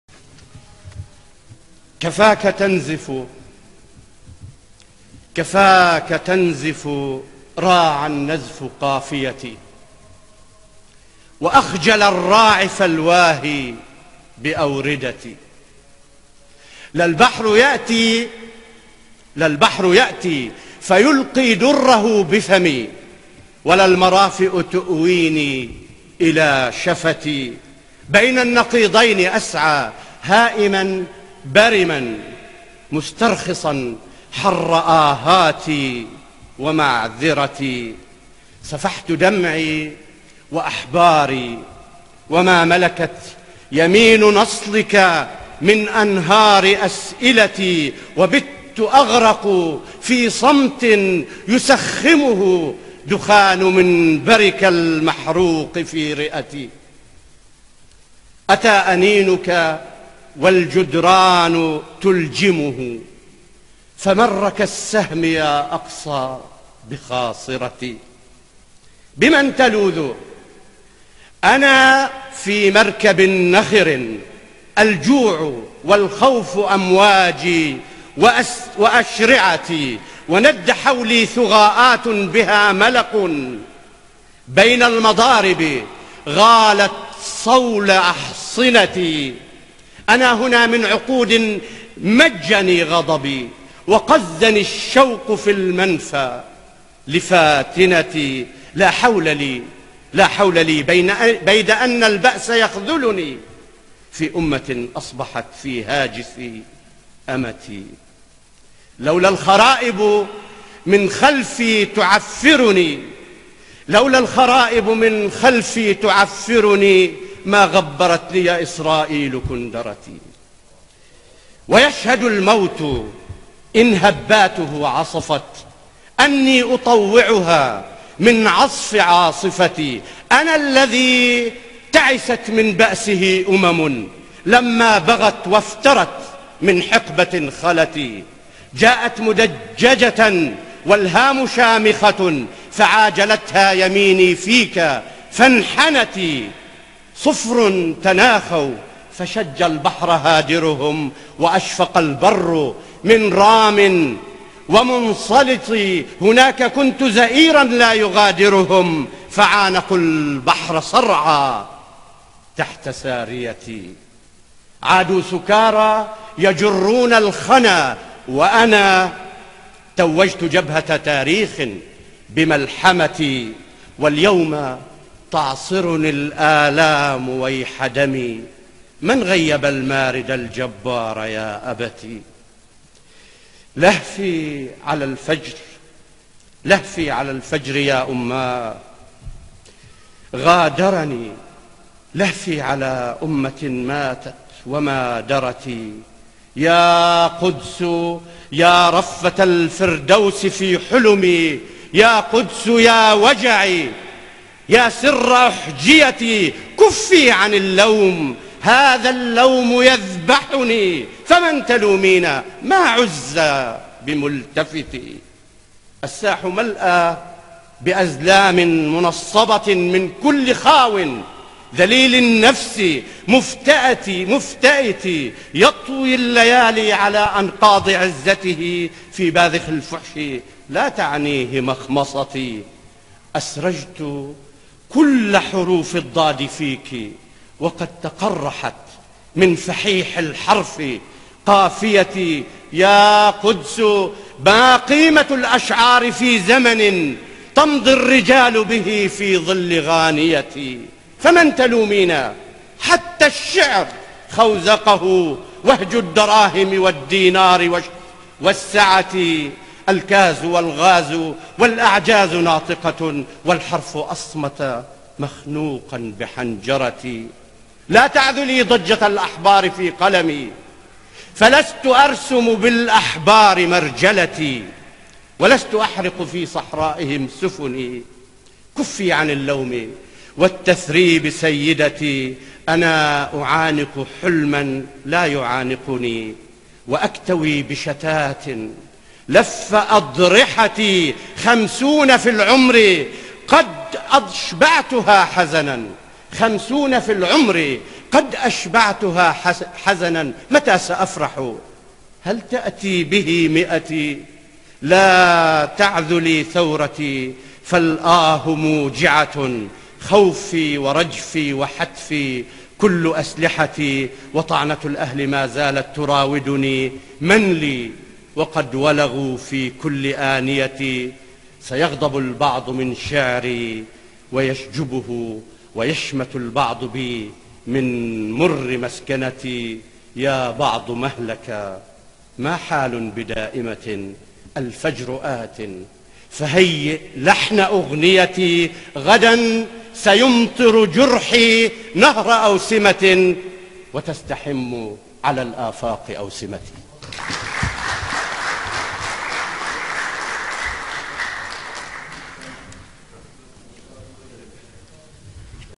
لا تعذلي ضجة الأحبار في قلمي - هلال الفارع
الشاعر هلال الفارع في منتدى النيلين للشعر العربي